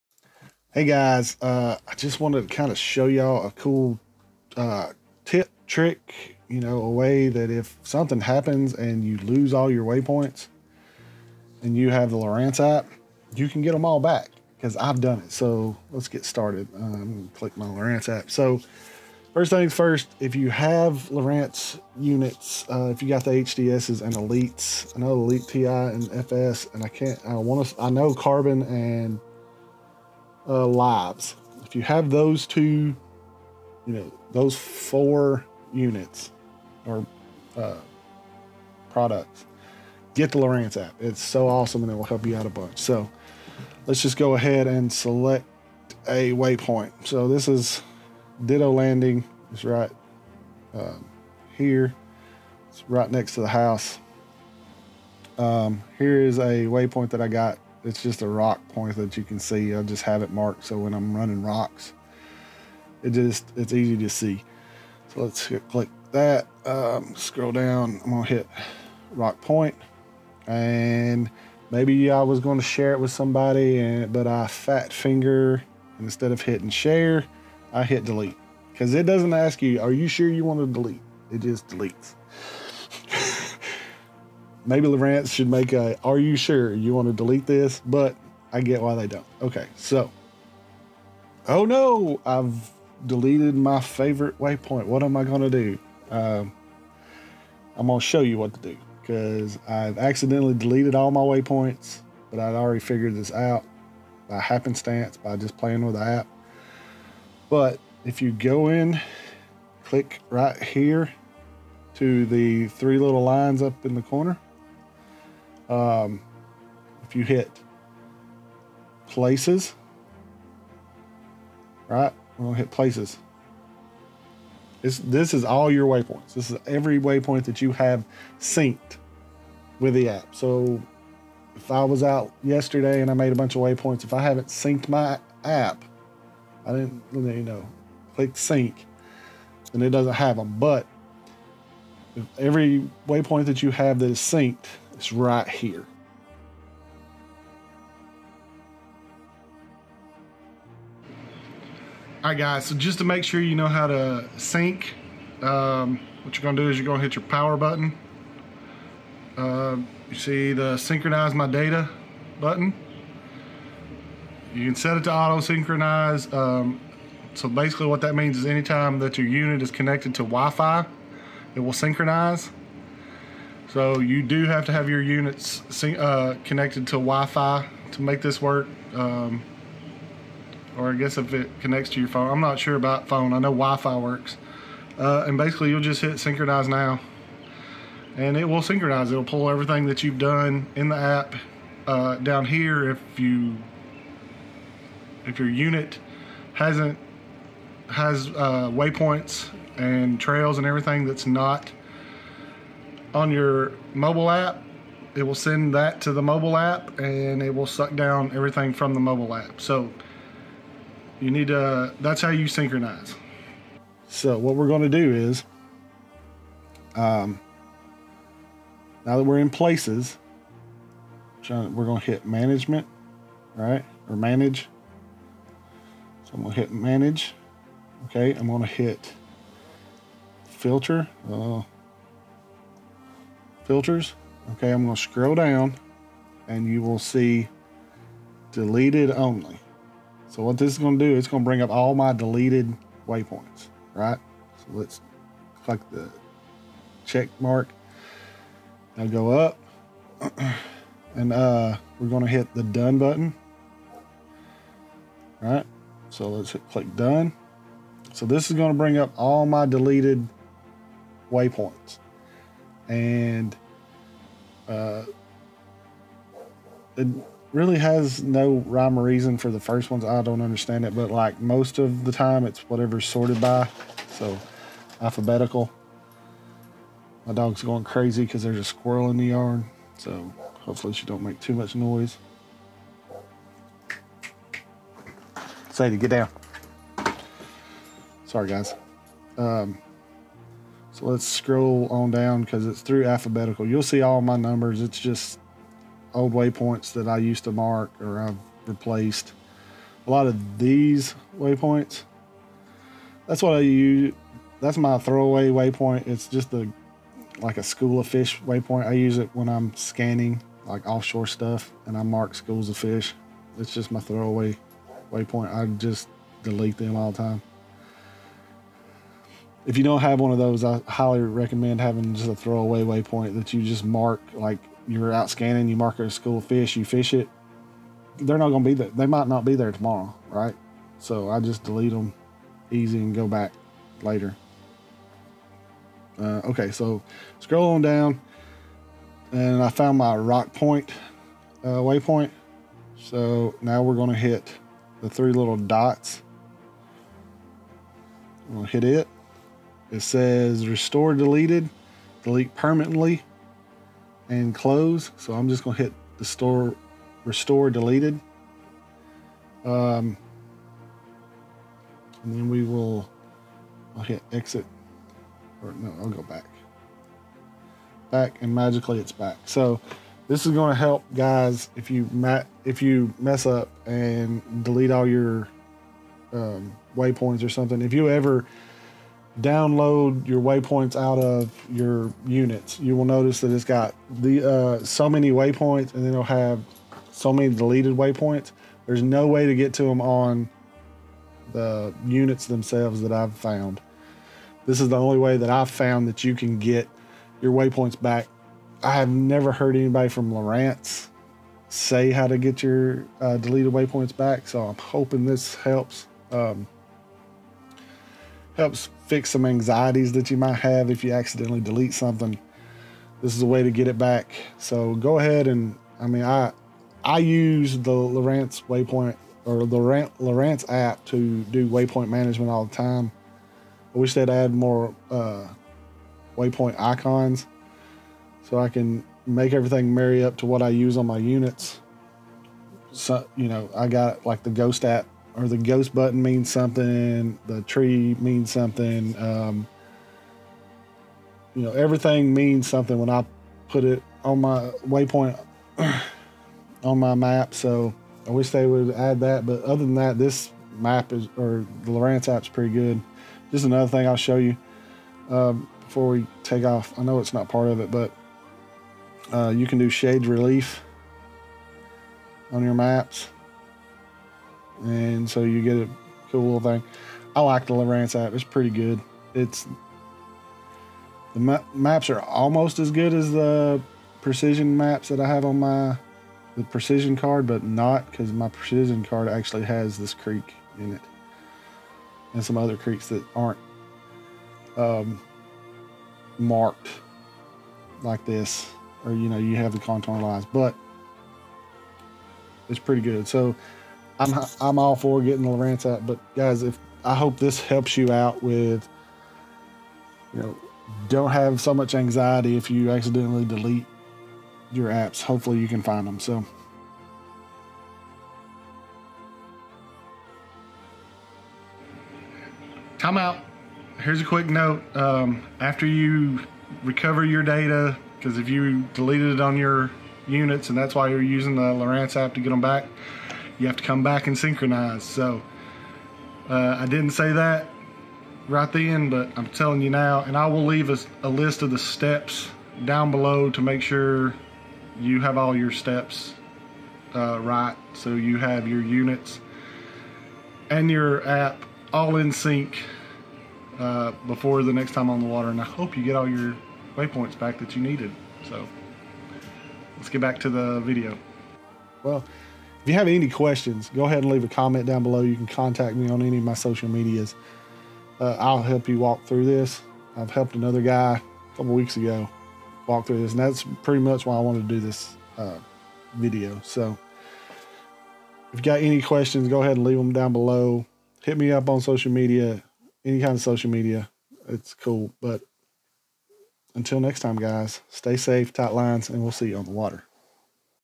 How to Restore DELETED Waypoints with the Lowrance App
In this video, I show you how to use the Lowrance App to get Deleted waypoints back. Lowrance stores the Deleted waypoints in a file but I have only found out how to access it from the App. The steps are listed below.

Step 1: If you have the Lowrance App, always download your waypoints to an SD card and keep a master file saved somewhere.
Step 2: Synch Units to the Mobile App. Hit the Power button and select Synch Data.
Step 3: Open App.
Step 4: Select Menu. (3 Lines in Top Left next to “Where to?”)
Step 5: Select Places
Step 6: Select Manage (Top Right Corner)
Step 7: Select Filters
Step 8: Select Deleted Only (it will get a checkmark beside it)
Step 9: Select Done.
Step 10: Scroll to find the waypoint. (Difficulty depends on your waypoint management style; names are easy, numbers are not.)
Step 11: Select the 3 dots to the Right of Waypoint Name.
Step 12: Select Restore Deleted.
(Follow Steps 10-12 for every waypoint you wish to Restore.)
Step 13: Synch Units to the Mobile App. Hit the Power button and select Synch Data.